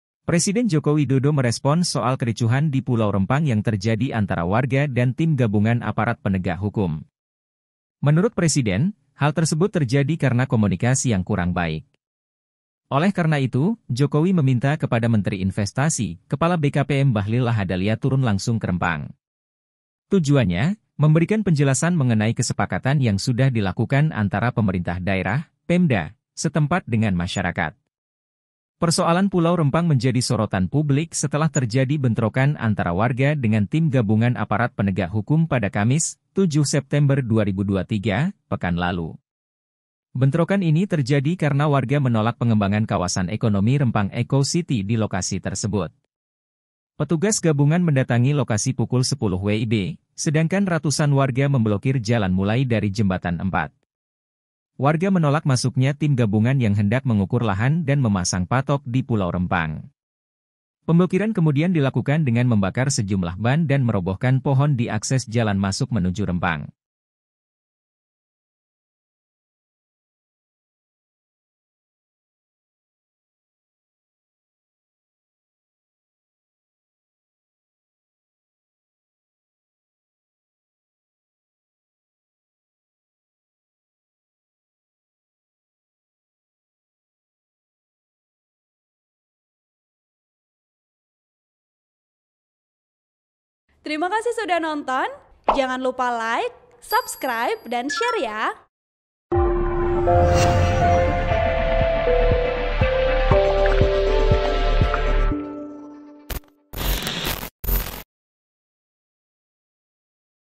Jokowi Tanggapi Kericuhan di Rempang Antara Aparat & Warga: Bentuk Komunikasi yang Kurang Baik
TRIBUN-VIDEO.COM - Presiden Joko Widodo merespons soal kericuhan di Pulau Rempang yang terjadi antara warga dan tim gabungan aparat penegak hukum.

Menurut Presiden, hal tersebut terjadi karena komunikasi yang kurang baik.

"Ya itu bentuk komunikasi yang kurang baik, kalau warga diajak bicara, diberikan solusi. Karena di situ sebenarnya sudah ada kesepakatan bahwa warga akan diberi lahan 500 meter plus bangunan tipe 45," kata Jokowi usai mengunjungi Pasar Kranggot di Banten, sebagaimana dilansir dari keterangan resmi pada Selasa (12/9/2023).

"Tetapi ini tidak dikomunikasikan dengan baik. Akhirnya menjadi masalah," ucap dia.

Oleh karena itu, Jokowi meminta kepada Menteri Investasi/Kepala BKPM Bahlil Lahadalia turun langsung ke Rempang.

Tujuannya, memberikan penjelasan mengenai kesepakatan yang sudah dilakukan antara pemerintah daerah (pemda) setempat dengan masyarakat.

"Menurut saya nanti mungkin besok atau lusa Menteri Bahlil (Menteri Investasi) akan ke sana untuk memberikan penjelasan mengenai itu," kata Jokowi.

Persoalan Pulau Rempang menjadi sorotan publik setelah terjadi bentrokan antara warga dengan tim gabungan aparat penegak hukum pada Kamis (7/9/2023) pekan lalu.

Bentrokan ini terjadi karena warga menolak pengembangan kawasan ekonomi Rempang Eco City di lokasi tersebut.

Petugas gabungan mendatangi lokasi pukul 10.00 WIB, sedangkan ratusan warga memblokir jalan mulai dari Jembatan 4.

Warga menolak masuknya tim gabungan yang hendak mengukur lahan dan memasang patok di Pulau Rempang.

Pemblokiran kemudian dilakukan dengan membakar sejumlah ban dan merobohkan pohon di akses jalan masuk menuju Rempang.

Artikel ini telah tayang di Kompas.com dengan judul "Soal Ricuh di Pulau Rempang, Jokowi: Bentuk Komunikasi yang Kurang Baik", Klik untuk baca